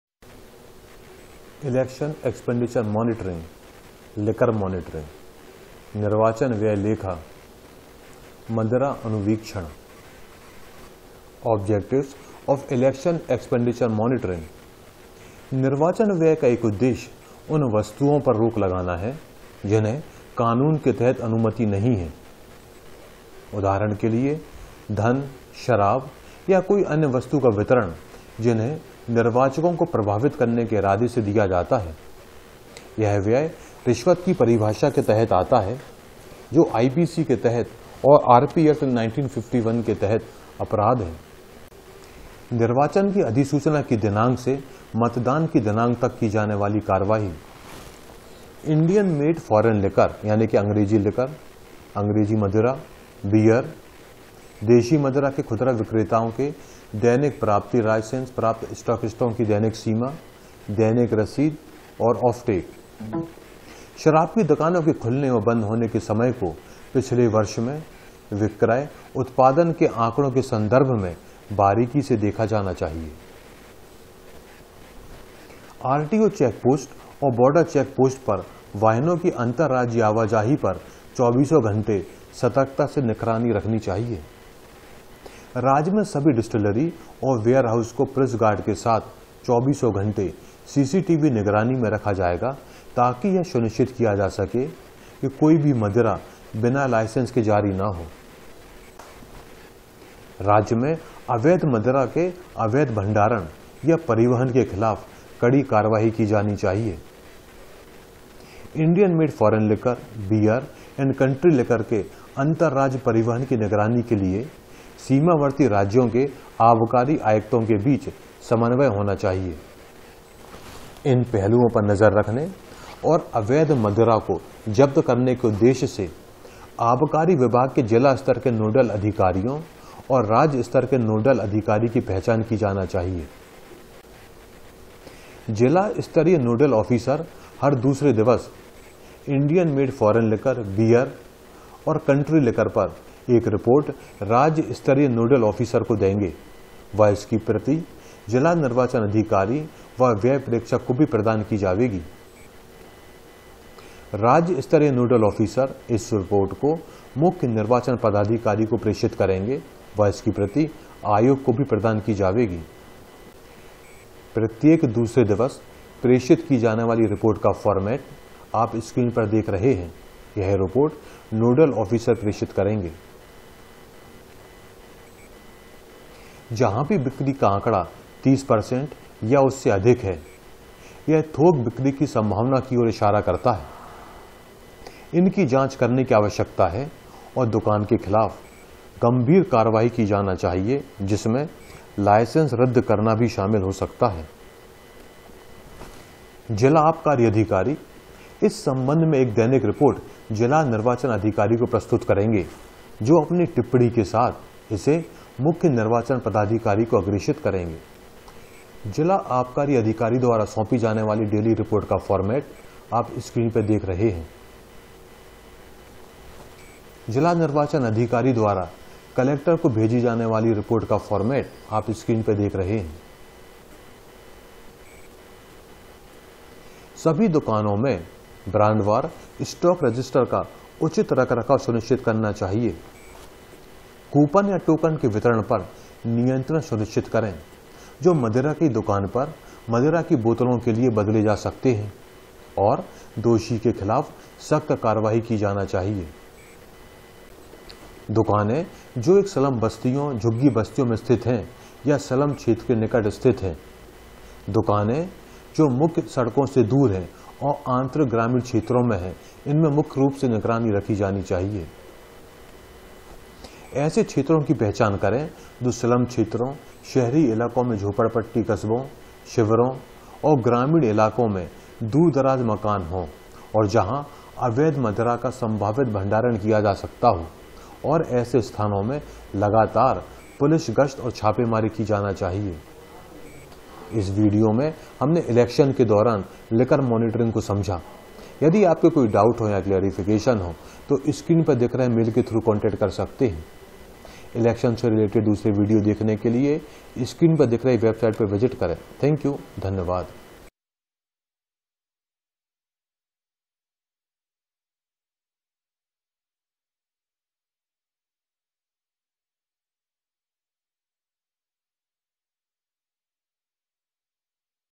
निर्वाचन व्यय लेखा – मदिरा अनुवीक्षण
निर्वाचन व्यय का एक उद्देश्य उन वस्तुओं पर रोक लगाना है जिन्हें कानून के तहत अनुमति नहीं है।
उदाहरण: - धन, शराब या कोई अन्य वस्तु का वितरण जिन्हें निर्वाचकों को प्रभावित करने के इरादे से दिया जाता है।
यह व्यय "रिश्वत" की परिभाषा के तहत आता है जो IPC के तहत और R.P. Act, 1951 के तहत अपराध है।
निर्वाचन की अधिसूचना की दिनॉक से मतदान की दिनॉक तक की जाने वाली कार्रवाई : -
IMFL / Beer / Country Liquor के खुदरा विक्रेताओं केदैनिक प्राप्ति, लाइसेंस प्राप्त स्टॉकिस्टों की दैनिक सीमा, दैनिक रसीद और ऑफ-टेक
शराब की दुकानों के खुलने और बंद होने के समय को पिछले वर्ष में, विक्रय/ उत्पादन के आंकड़ों के संदर्भ में बारीकी से देखा जाना चाहिए।
RTO चेक पोस्ट और बॉर्डर चेक पोस्ट पर वाहनों की अंतर-राज्य आवाजाही पर चौबीसों घंटे सतर्कता बरती जाना चाहिए ।
  राज्य में सभी डिस्टलरी और वेयरहाउस को पुलिस गार्ड के साथ 24X 7 सीसीटीवी निगरानी में रखा जाएगा, ताकि यह सुनिश्चित किया जा सके कि कोई भी मदिरा बिना लाइसेंस के जारी न हो।
राज्य में अवैध मदिरा के अवैध भंडारण या परिवहन के खिलाफ कड़ी कार्रवाई की जानी चाहिए।
IMFL, Beer and Country Liquor के अंतर-राज्य परिवहन की निगरानी के लिए सीमावर्ती राज्यों के आबकारी आयुक्तों के बीच समन्वय होना चाहिए ।
इन पहलुओं पर नजर रखने और अवैध मदिरा को जब्त करने के उद्देश्य से आबकारी विभाग के जिला स्तर के नोडल अधिकारियों और राज्य स्तर के नोडल अधिकारी की पहचान की जानी चाहिए।
जिला स्तरीय नोडल ऑफिसर हर दूसरे दिवस IMFL, बीयर और देशी मदिरा पर एक रिपोर्ट राज्य स्तरीय नोडल ऑफिसर को देंगें व इसकी प्रति जिला निर्वाचन अधिकारी व व्यय प्रेक्षक को  भी प्रदान की जावेगी ।
राज्य स्तरीय नोडल ऑफिसर इस रिपोर्ट को मुख्य निर्वाचन पदाधिकारी को प्रेषित करेंगें व इसकी प्रति आयोग को  भी प्रदान की जावेगी ।